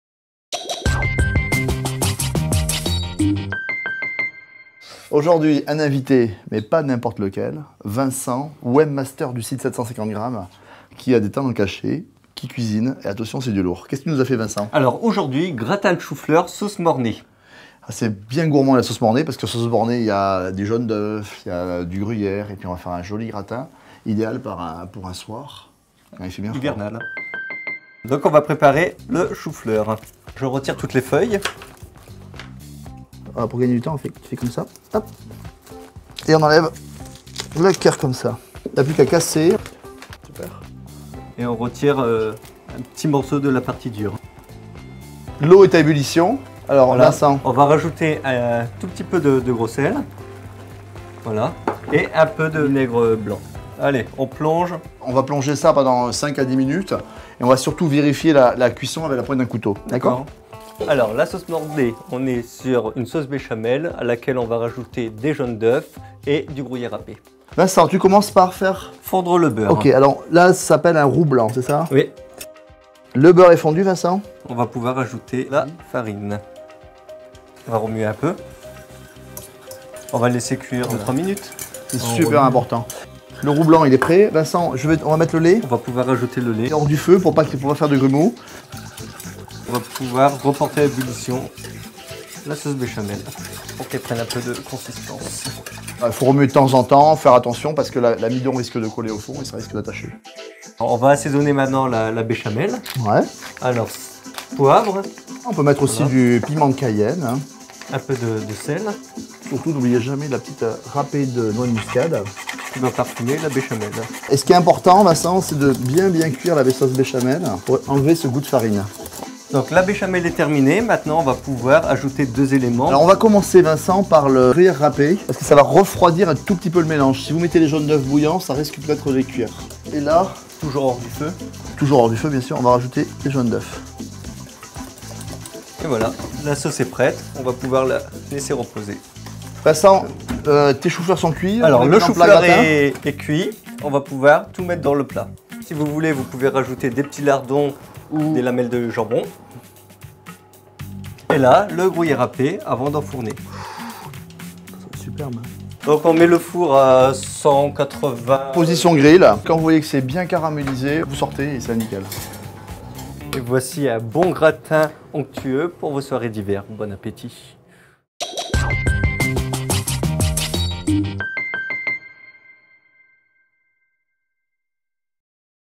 Recette de gratin de chou fleur - 750g
Un gratin de chou-fleur pour un plat simple et rapide. Le gratin de chou-fleur et sa sauce béchamel ravira même les enfants !

Une recette proposée par Chef Damien et Vincent de 750 Grammes.

Ingrédients pour le gratin de chou-fleur :
1 chou-fleur
Gros sel
Vinaigre
50g de beurre
50g de farine
40 cl de lait
2 jaunes d'œufs
200g de gruyère râpé

Et toutes nos vidéos sur notre chaîne 750 Grammes